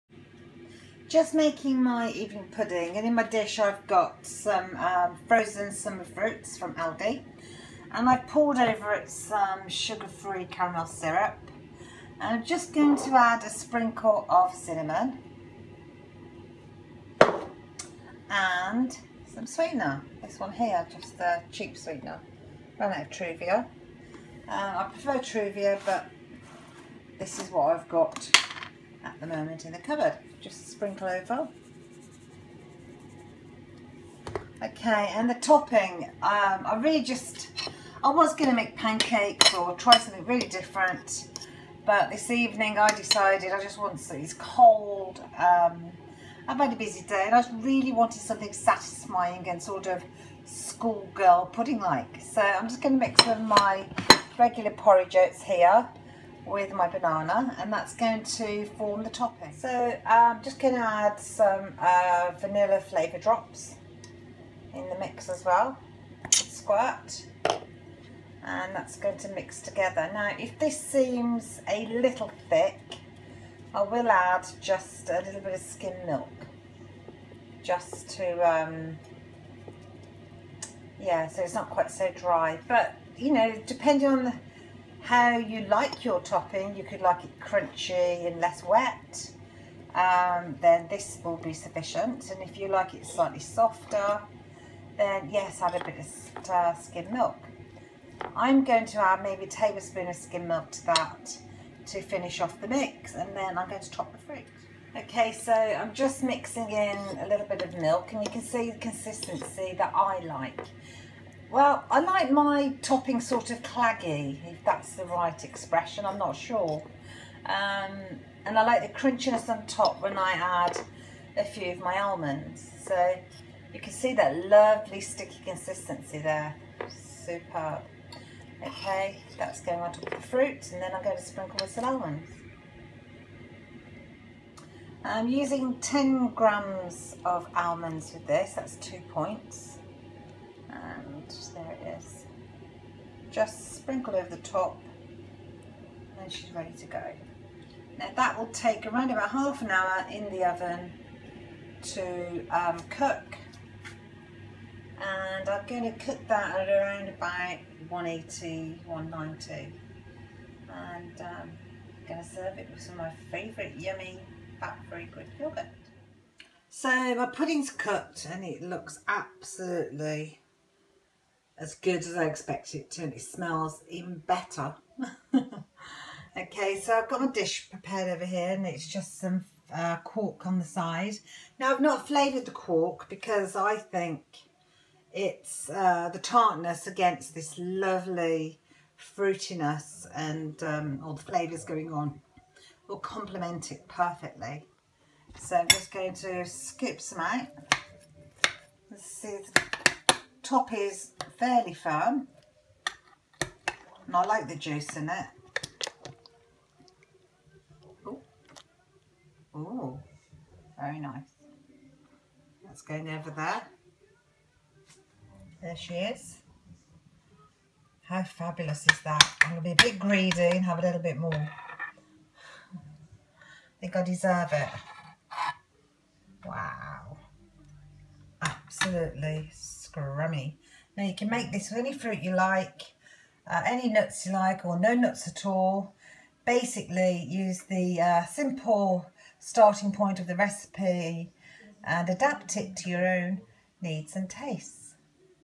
Summer fruit and banana mock crumble
Mash banana and mix with approx 30 g oats , vanilla flavouring, cinnamon , if a little dry add splash of skimmed milk

Pop frozen fruit in dish , pour over sugar free caramel coffee syrup , top with mix and 10 g almonds 2sp and bake approx 30-40
Mins 180